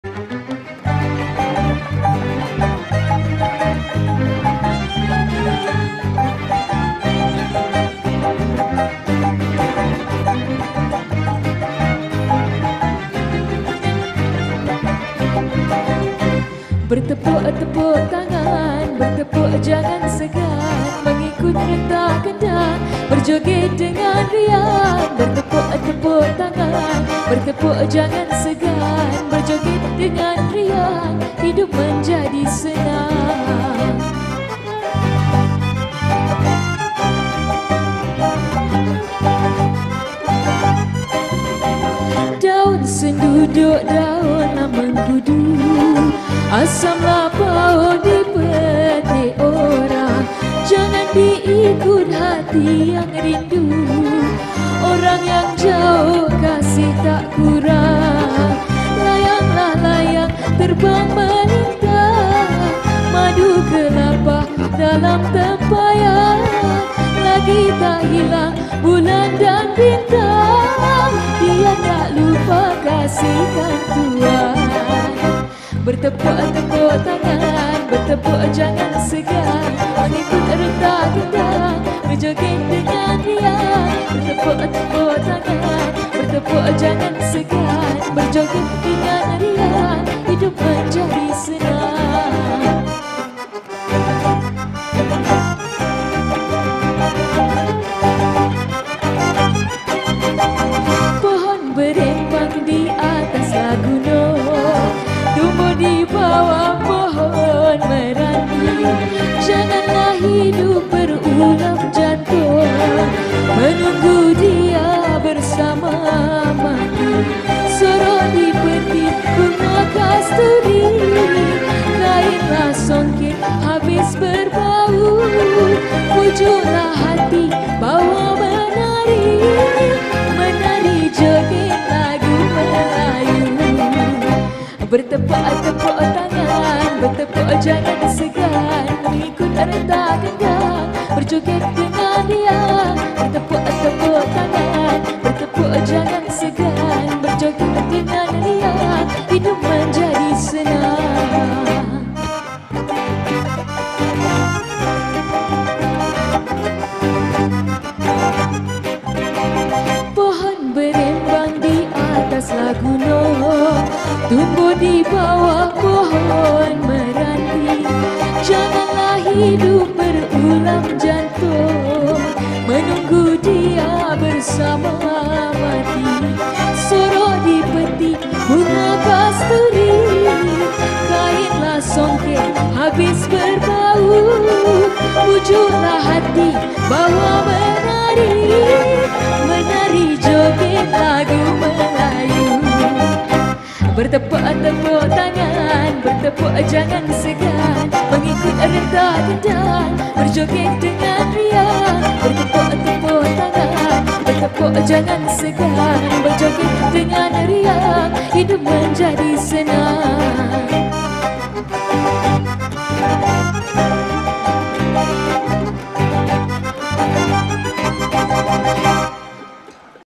Joget Berhibur di Bora Ombak Putrajaya
Persembahan Jamilah dan Keroncong Jamilah Music Heritage di Majlis Perkahwinan Syafuq dan Syahira di Bora Ombak pada 26 Mac 2021